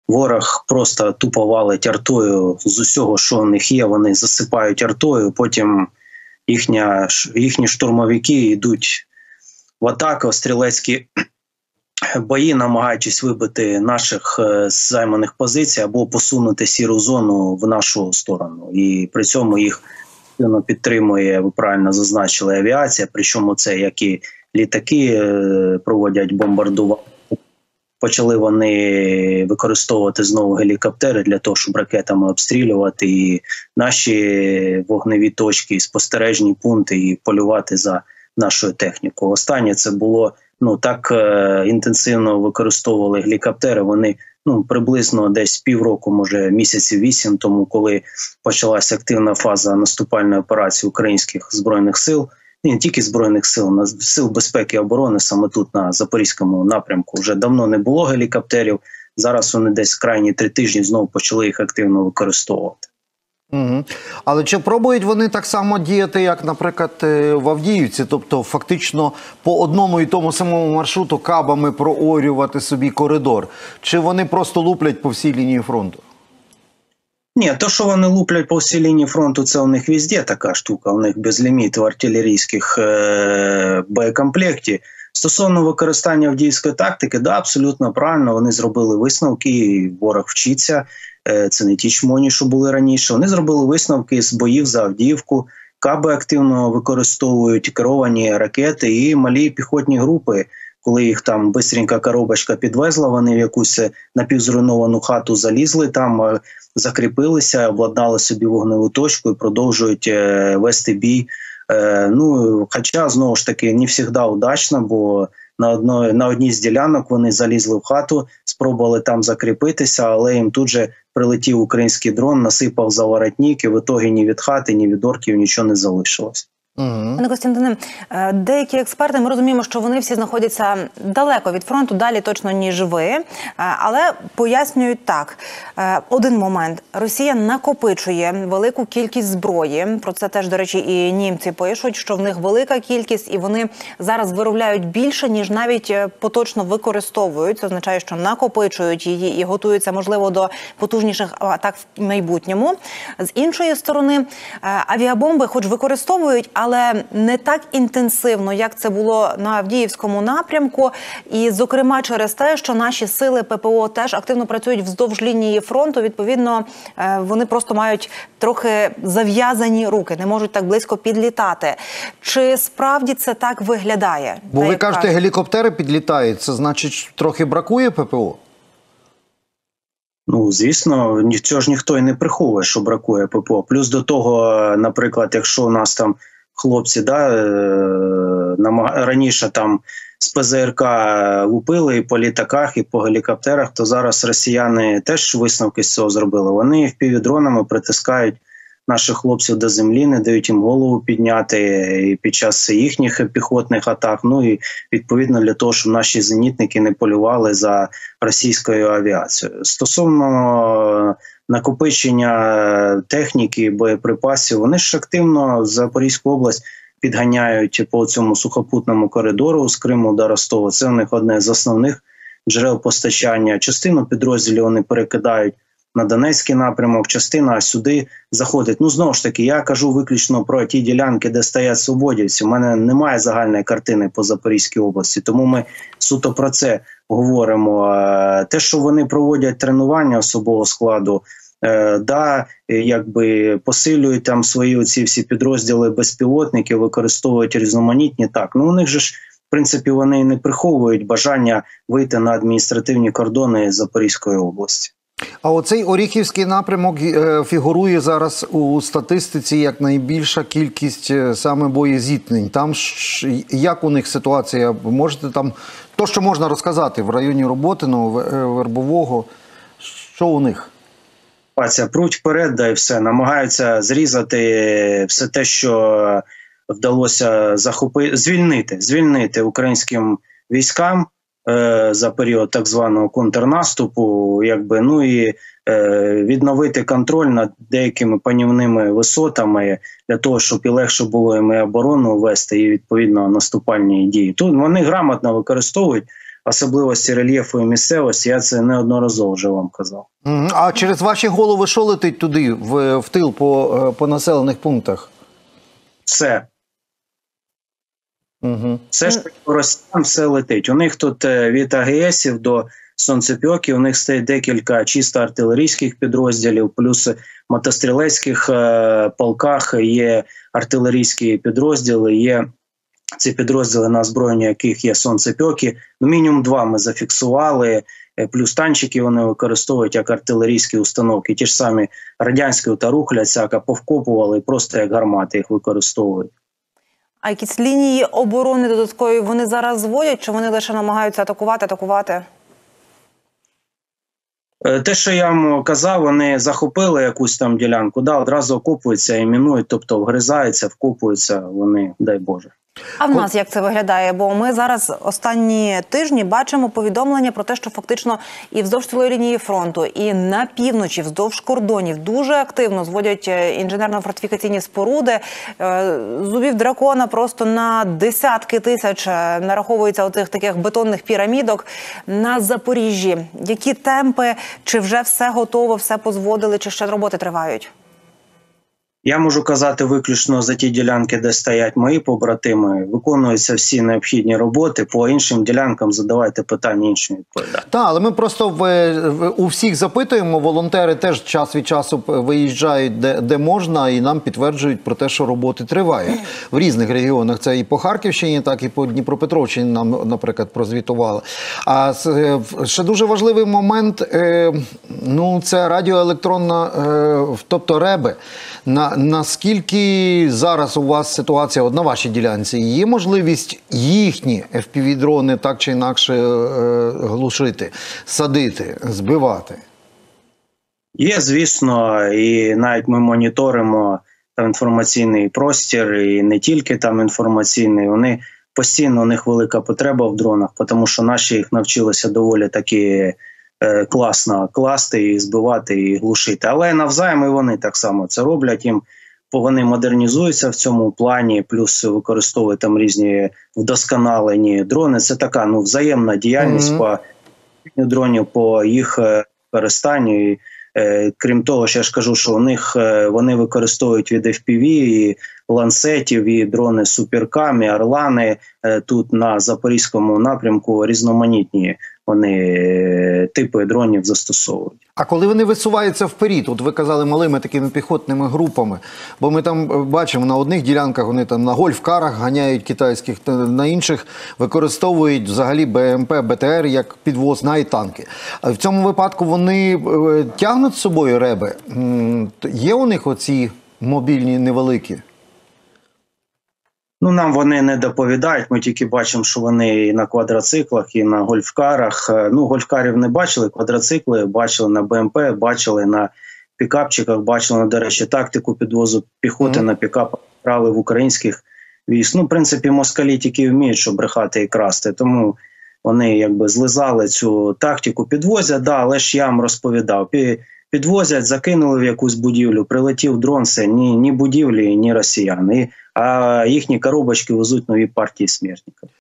Запорізький напрямок: Ворог постійно валить з усього, намагаючись посунути сіру зону — Денисов
Запоріжжя: Російські окупанти безупинно валять з усіх видів зброї, що мають. До всього, після кілька місячної паузи, московити почали активно використовувати гелікоптери. Ворог не полишає спроб посунути "сіру зону" — речник "Легіону Свободи" Костянтин Денисов з оглядом фронтової ситуації станом на 31 березня. З етеру телеканалу "Еспресо"

❗️ Реквізити для допомоги бійцям «Легіону Свободи» 👇👇👇
✓ Посилання на Банку:
💳 Картка Монобанк: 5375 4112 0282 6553
💳 Картки Приватбанк:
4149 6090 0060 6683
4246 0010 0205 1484